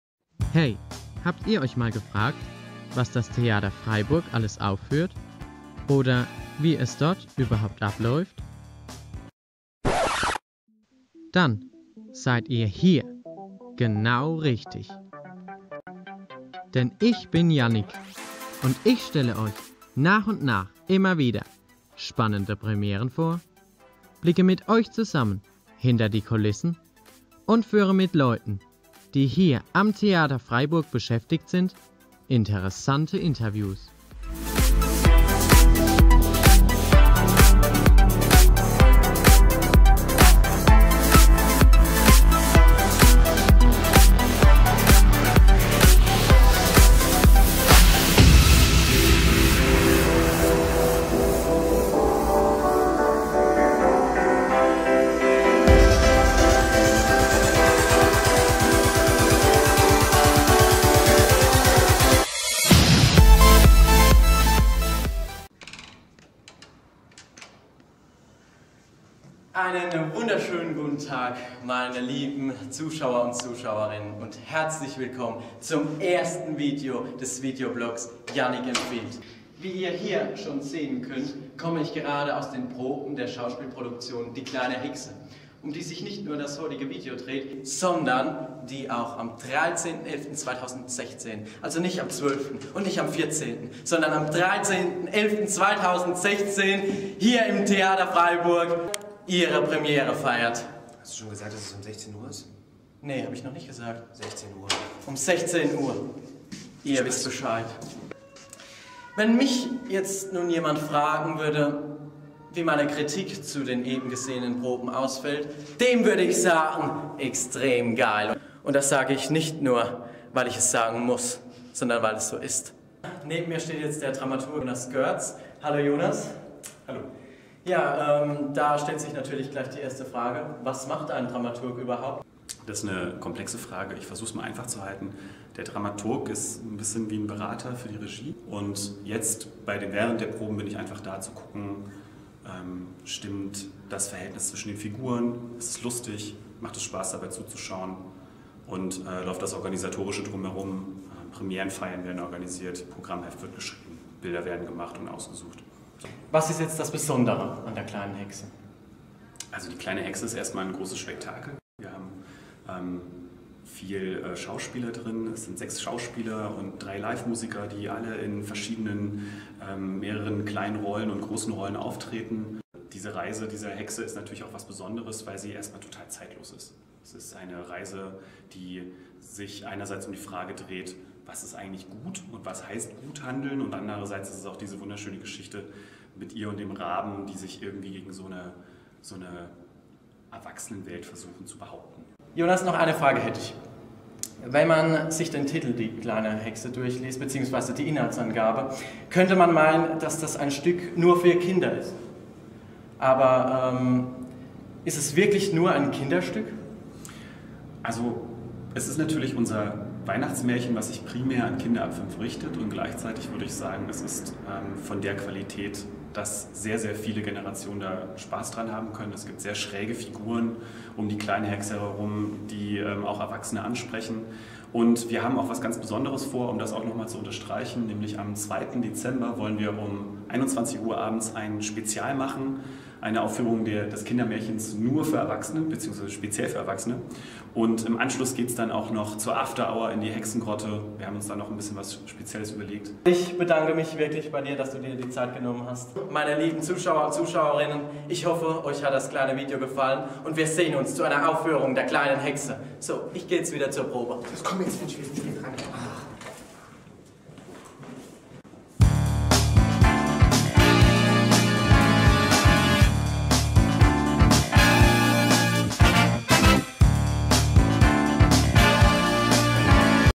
Theater Freiburg: Jannic empfiehlt #1 - Die kleine Hexe
"Jannic empfiehlt" ist unser neuer Videoblog. Unser FSJler Jannic berichtet für euch von Proben, führt interessante Interviews und gibt euch exklusive Einblicke hinter die Kulissen. In dieser Folge: Die kleine Hexe.